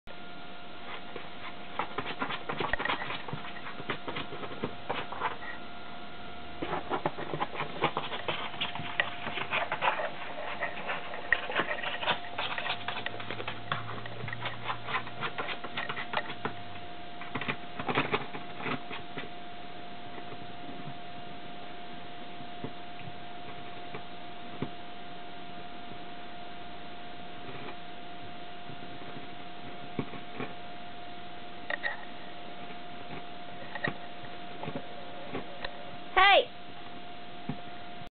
Ginger our crazy Min Pin / Border terrier puppy mad at box
Our 10 week old puppy got mad at a cardboard box.